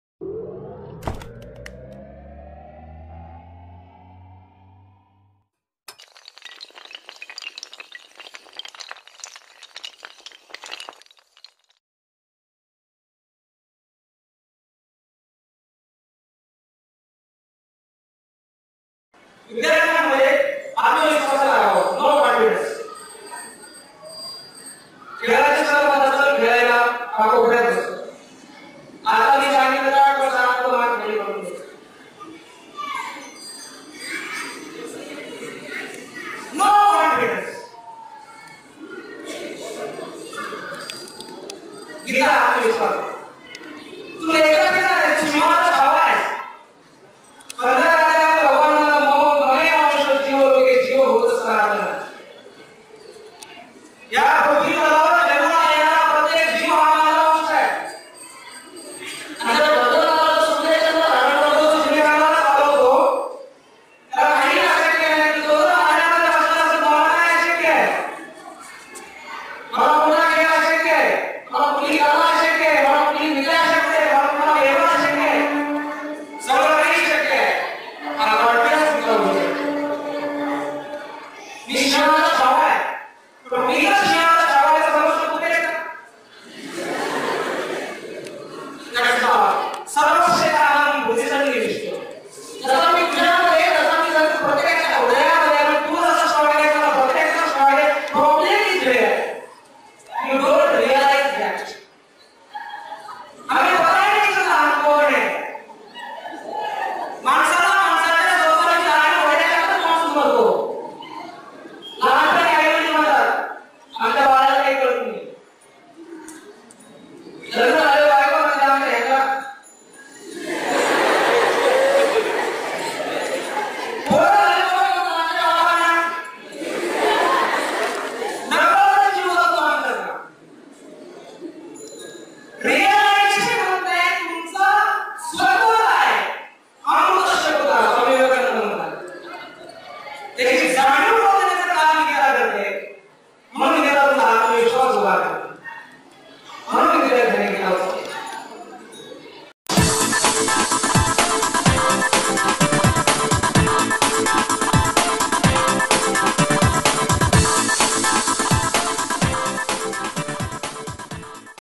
आत्मविश्वास वाढवा।श्रीमद भगवद्गीता। marathi motivation
आत्मविश्वास ही यशाची गुरुकिल्ली आहे. तुमचा आत्मविश्वास वाढवण्याचा गुरुमंत्र गीता देते.पहा हा व्हिडिओ